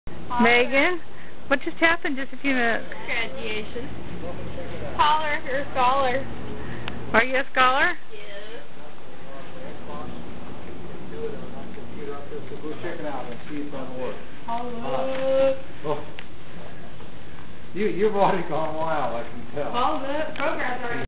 Holla if you a Scholla...
After Megans graduation she took all her honors tassels and wore them in a more colorful way